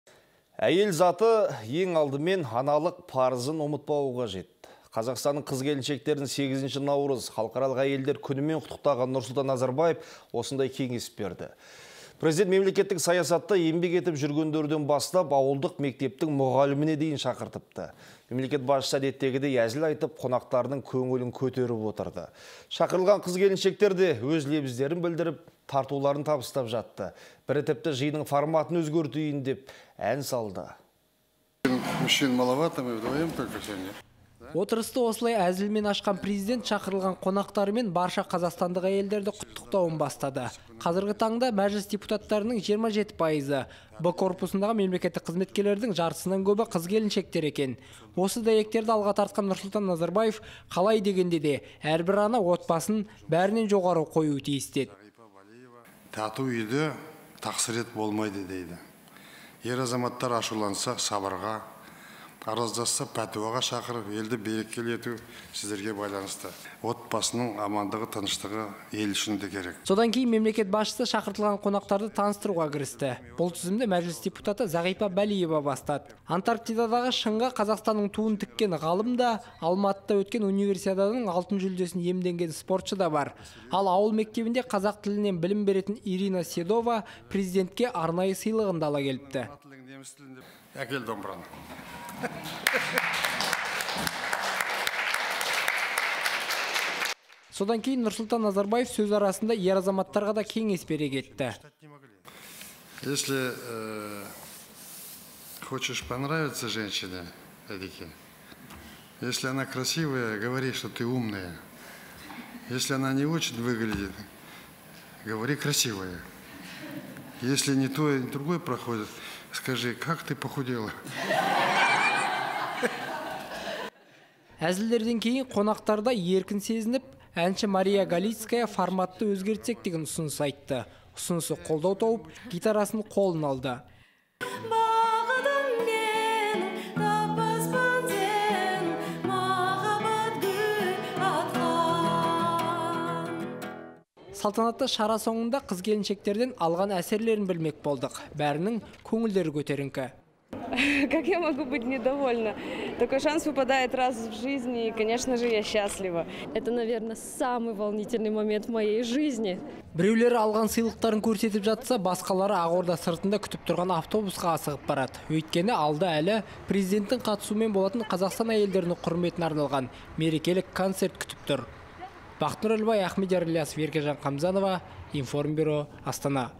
Президент әйелдер мерекесіне орай қыз-келіншектермен кездесу өткізді
Әйел заты - ең алдымен аналық парызын ұмытпауы қажет. Қазақстанның қыз-келіншектерін 8 наурыз халықаралық әйелдер күнімен құттықтаған Нұрсұлтан Назарбаев осындай кеңес берді. Президент мемлекеттік саясатта еңбек етіп жүргендерден бастап, ауылдық мектептің мұғаліміне дейін шақырыпты. Мемлекет басшысы әдеттегідей әзіл айтып, қонақтарының көңілін көтеріп отырды. Шақырылған қыз-келіншектер де өз лебіздерін білдіріп, тартуларын табыстап жатты. Бірі тіпті, жиынның форматын өзгертейін деп ән салды.